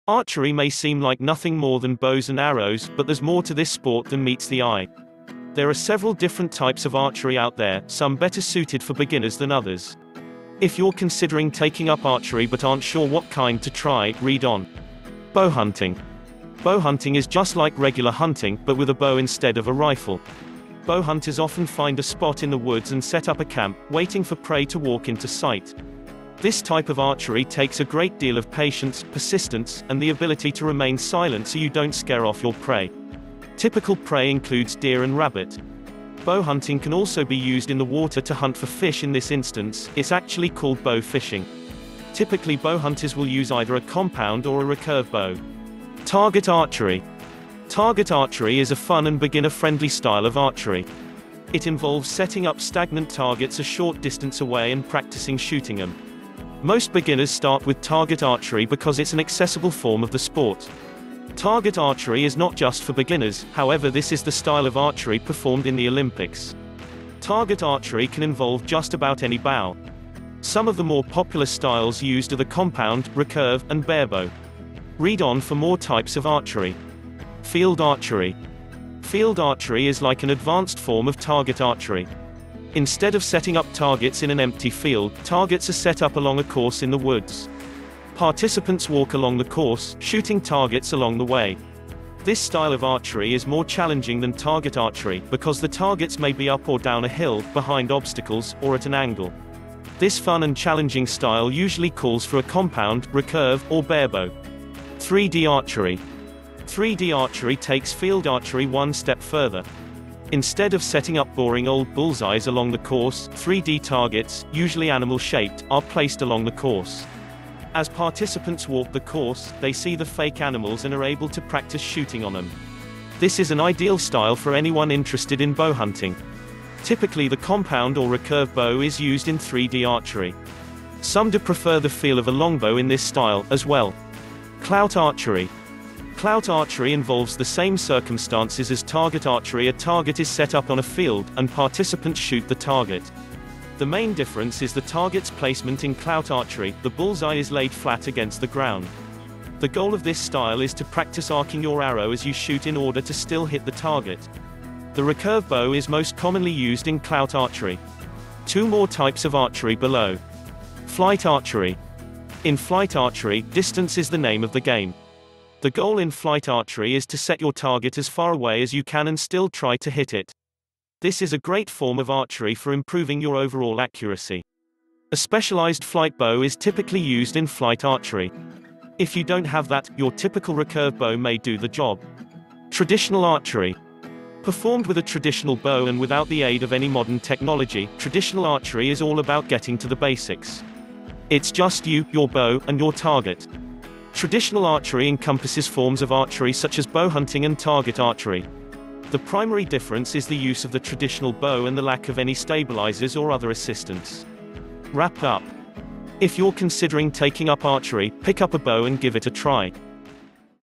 Types of Archery
Archery may seem like nothing more than bows and arrows, but there’s more to this sport than meets the eye. There are several different types of archery out there, some better suited for beginners than others.